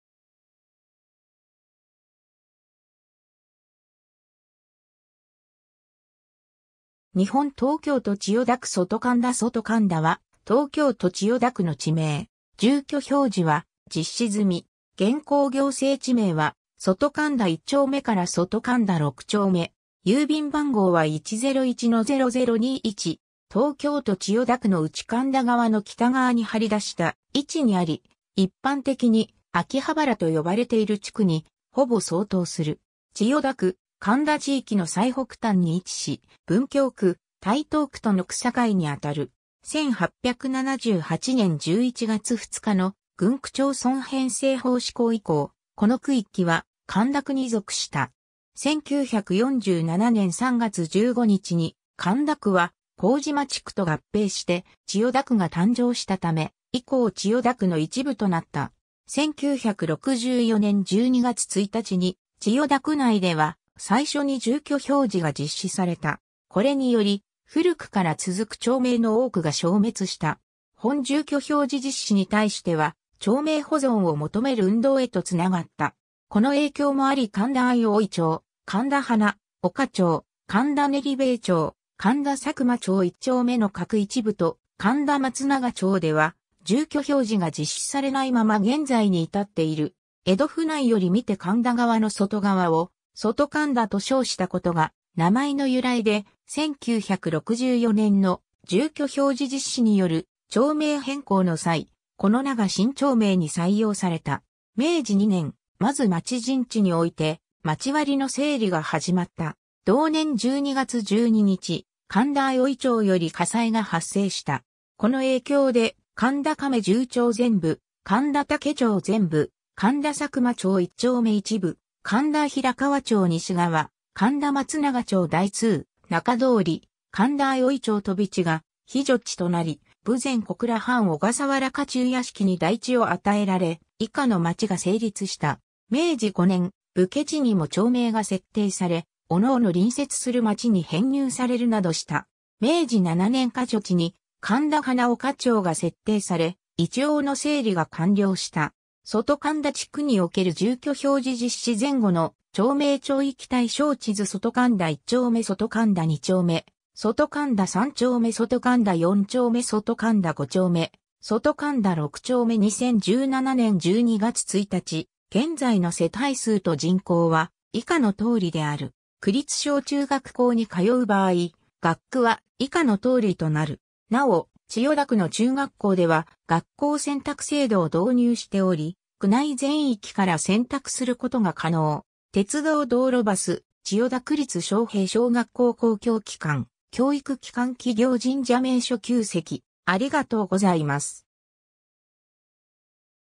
外神田
上野・浅草副都心
日本 」 東京都 」 千代田区 」 外神田 外神田（そとかんだ）は、東京都千代田区の地名。
住居表示は実施済み。
現行行政地名は外神田一丁目から外神田六丁目。
 東京都千代田区のうち神田川の北側に張り出した位置にあり、一般的に秋葉原と呼ばれている地区にほぼ相当する。
千代田区・神田地域（旧：神田区）の最北端に位置し、文京区（湯島）、台東区（秋葉原・上野）との区境に当たる。
 1878年11月2日の郡区町村編制法施行以降、この区域は神田区に属した。
1947年3月15日に神田区は麹町区と合併して千代田区が誕生したため、以降千代田区の一部となった。
1964年12月1日に、千代田区内では最初に住居表示が実施された。
これにより、古くから続く町名の多くが消滅した。
 本住居表示実施に対しては、町名保存を求める運動へとつながった（さらに、区議会が全会一致で住居表示実施案の見直しを採択したり、区長の辞任騒動にまで発展した）。
この影響もあり神田相生町・神田花岡町・神田練塀町・神田佐久間町一丁目の各一部と神田松永町では、住居表示が実施されないまま現在に至っている。
 江戸府内より見て神田川（外堀）の外側を「外神田」（内側は「内神田」）と称したことが名前の由来で、1964年の住居表示実施による町名変更の際、この名が新町名に採用された。
 明治2年（1869年）、まず町人地において町割の整理が始まった。
 同年12月12日、神田相生町より火災が発生した。
この影響で、神田亀住町全部、神田竹町全部、神田佐久間町一丁目一部（旧神田柳屋敷含む）、神田平河町西側、神田松永町大通・中通、神田相生町飛地が火除地となり、豊前小倉藩小笠原家中屋敷に代地を与えられ、以下の町が成立した。
 明治5年（1872年）、武家地にも町名が設定され、各々隣接する町に編入されるなどした。
 明治7年（1874年）火除地に神田花岡町が設定され、一応の整理が完了した。
 外神田地区における住居表示実施前後の町名町域対照地図 外神田一丁目 外神田二丁目 外神田三丁目 外神田四丁目 外神田五丁目 外神田六丁目 2017年（平成29年）12月1日現在の世帯数と人口は以下の通りである。
なお、千代田区の中学校では学校選択制度を導入しており、区内全域から選択することが可能。
 鉄道 道路 バス 千代田区立昌平小学校（2018年2月3日撮影） 公共機関 教育機関 企業 神社 名所 旧跡